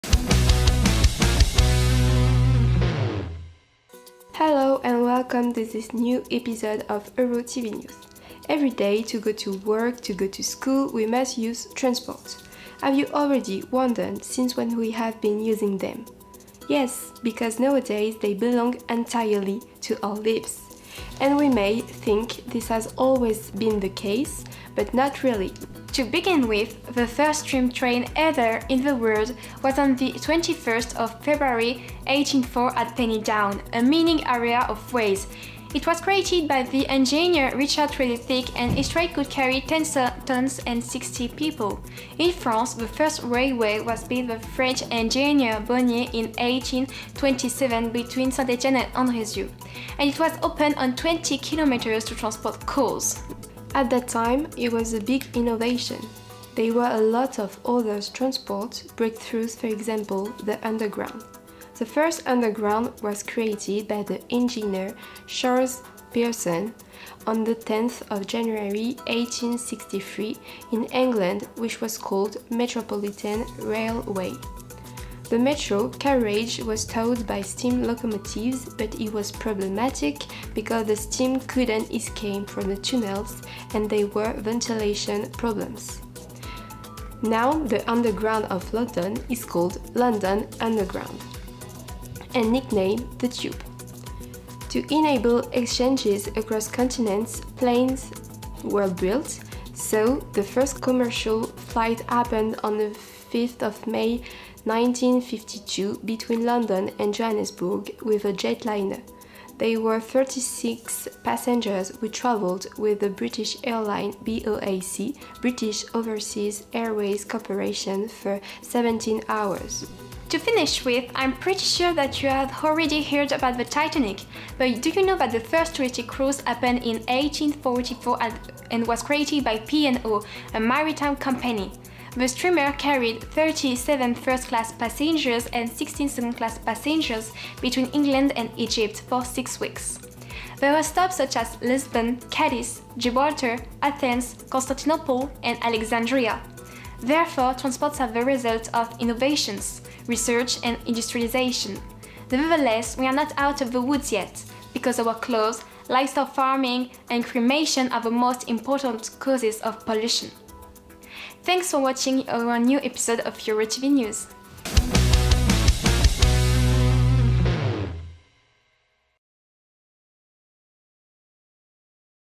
EURO TV NEWS: The History of Transportation
hosted by Eva and Kayla